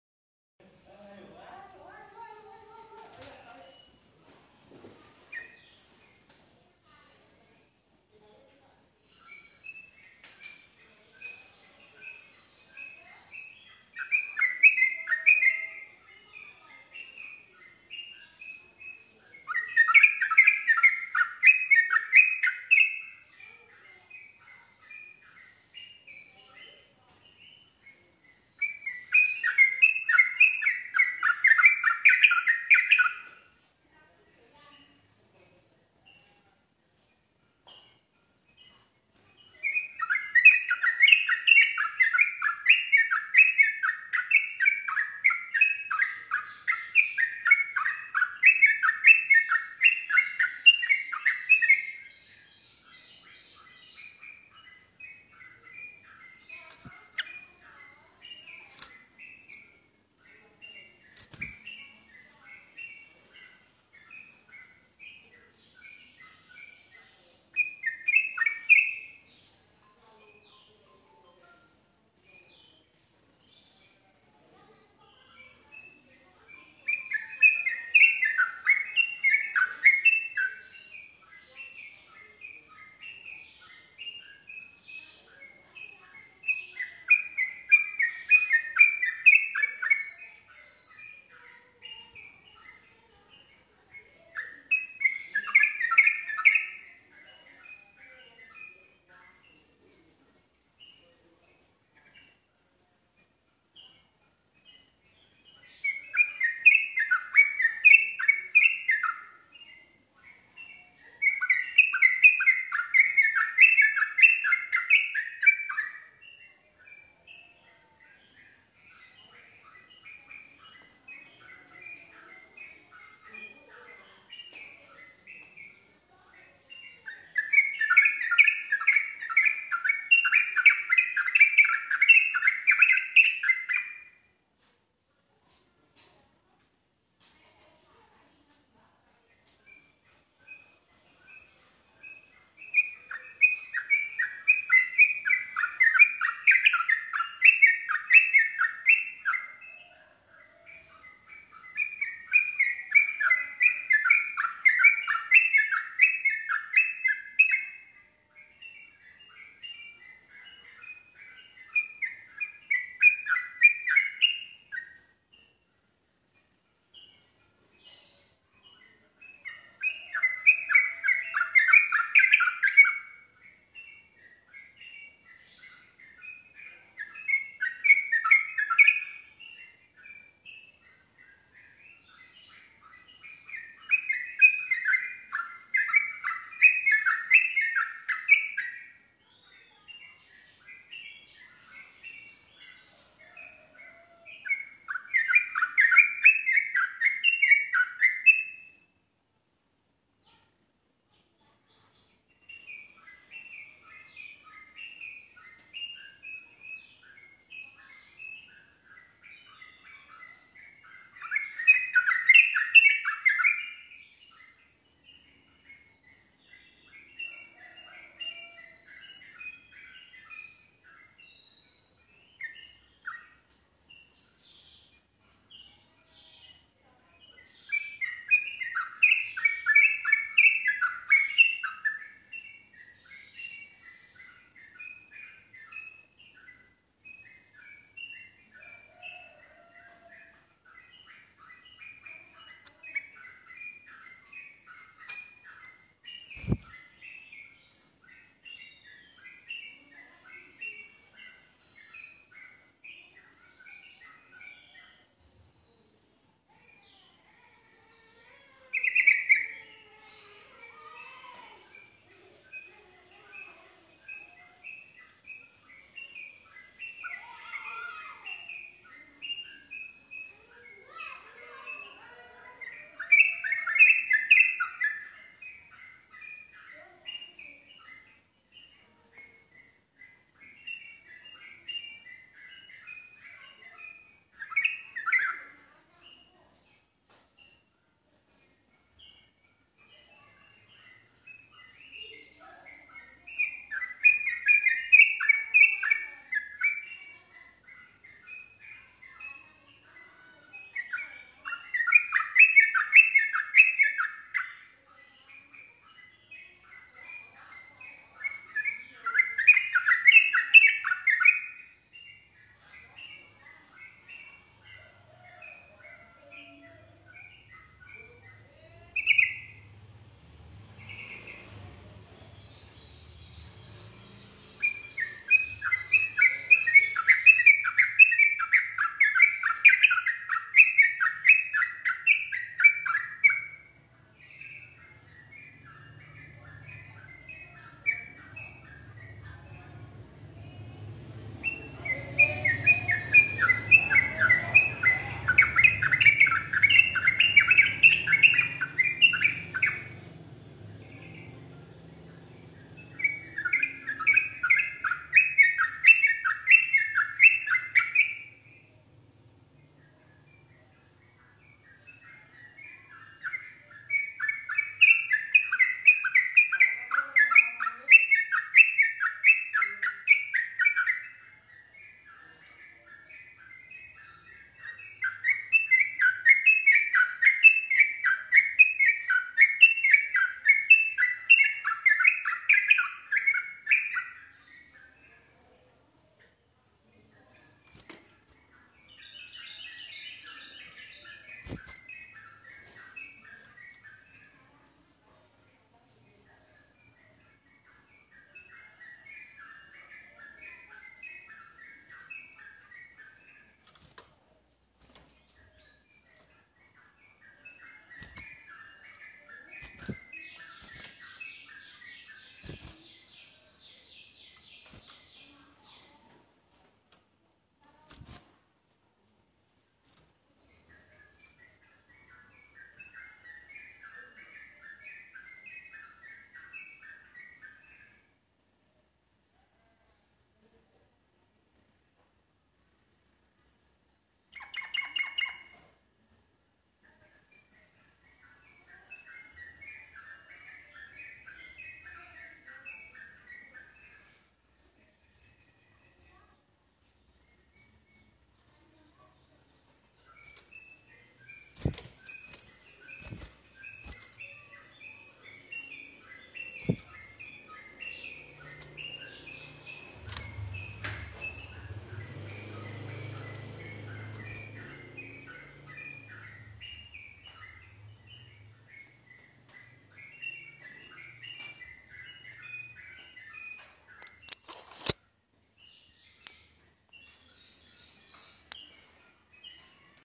respon cepat untuk Trucuk bahan macet langsung terpancing nyaut ikut nropel
Vidio trucuk memancing trucuk lain mengikuti terpancing langsung ikut ropel